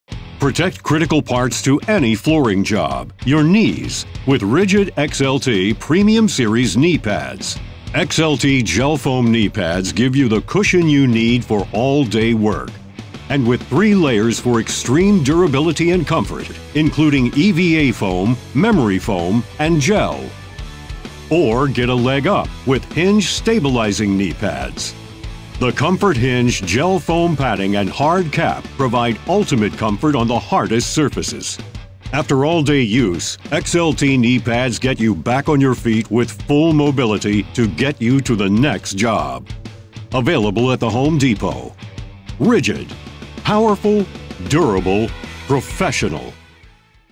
RIDGID XLT Knee Pads
RIDGID XLT Knee Pads bring all day comfort on the jobsite. Gel Foam padding provides 3 layers of comfort to ensure you can spend more time getting the job done and less time resting your knees. Watch the video above to see all options and features.